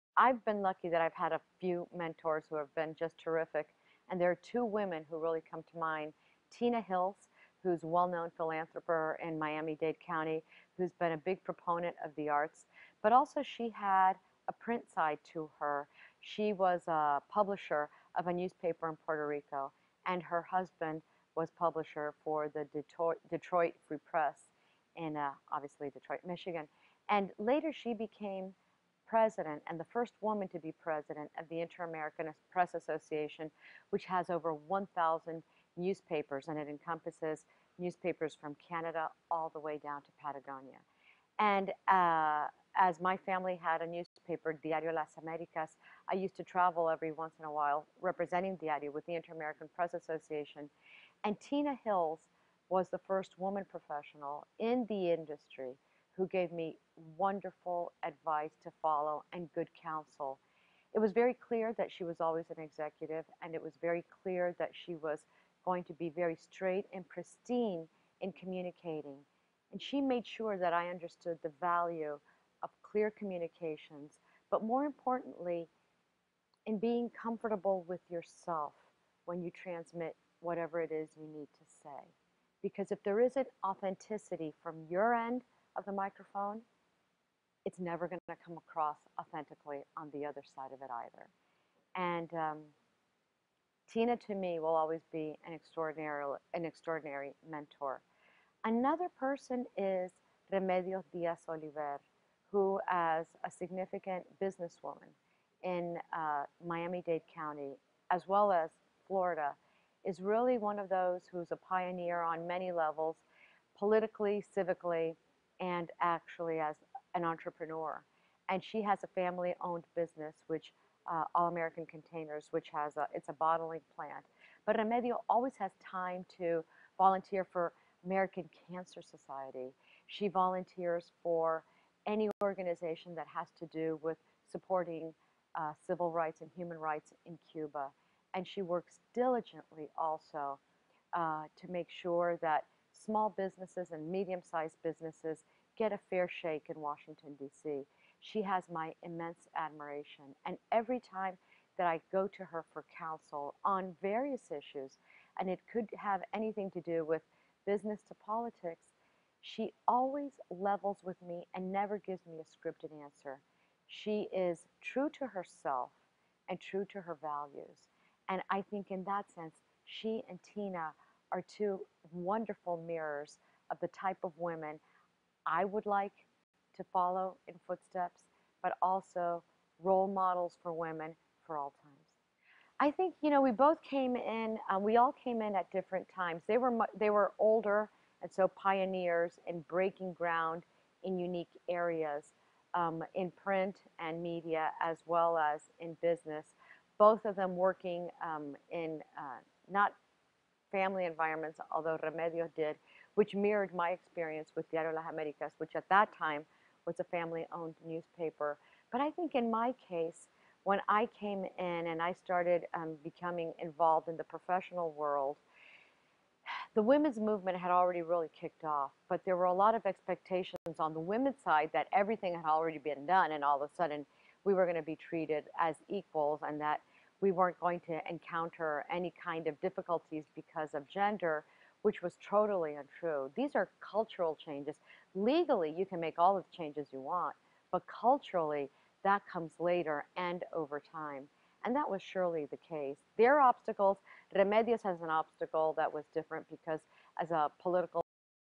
Makers Profile - Helen Aguierre Ferré
Award-winning bilingual journalist, co-host of Prohibido Callarre on Univision Radio and host of Issues, public television public affairs program. Helen is also the Opinion Page Editor and weekly columnist of Diario Las Americas.